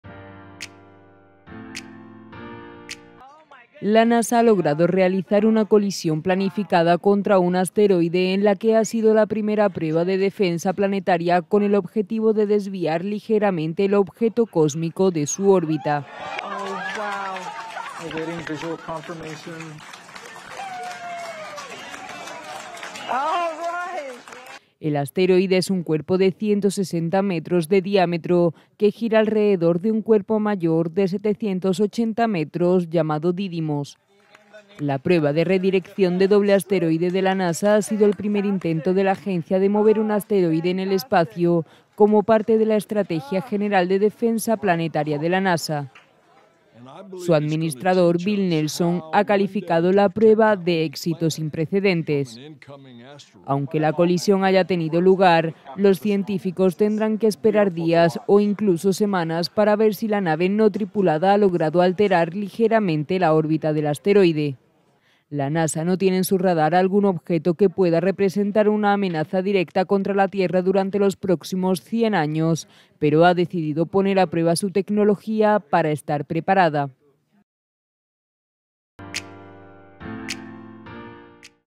Ahora en DF | Misión histórica de la NASA para desviar un asteroide: "Tenemos impacto"
La NASA logró realizar una colisión planificada contra un asteroide en la que fue la primera prueba de defensa planetaria con el objetivo de desviar ligeramente el objeto cósmico de su órbita.